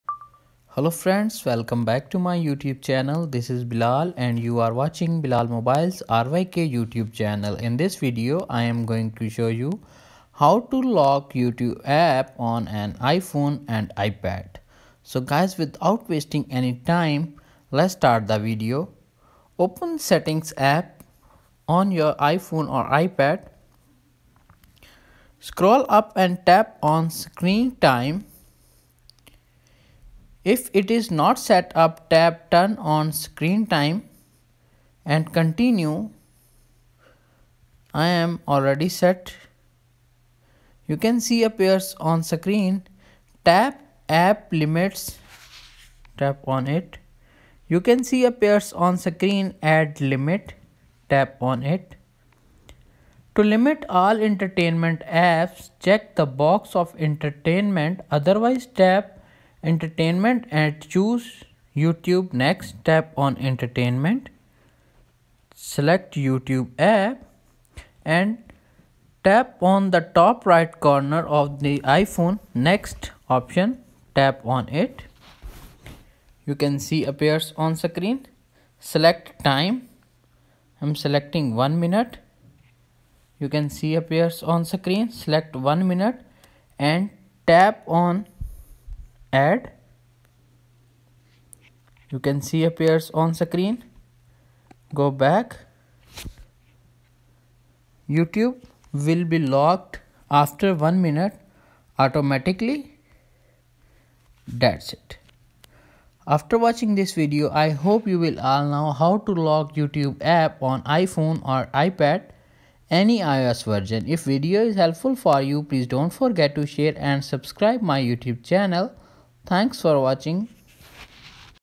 How To Lock Youtube App On iPhone Or iPad ( New 2022 )
Open Settings App On iPhone Or oPad
Tap On Screen Time
Turn on Screen Time
Tap On App Limit
Tap On Add Limit
Tap Emtertainment and Select Youtube App
tap On Next
Set The Time Limit
Tap On Add

Thats IT